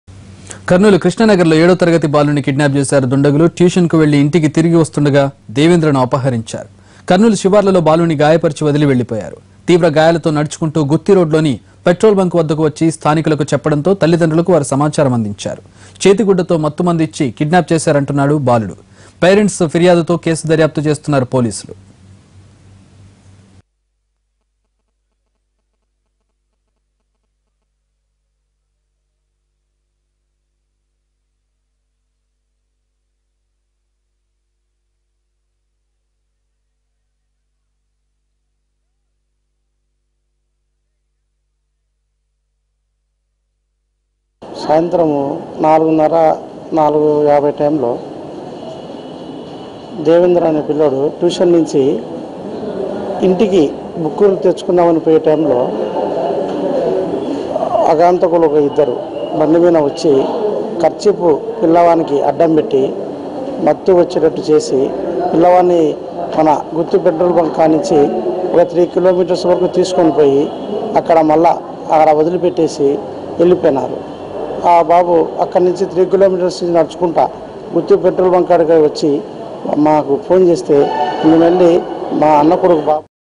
7th Class Student abducted And Seriously Wounded In Kurnool District | NTV
7th Class Student abducted And Seriously Wounded In Kurnool District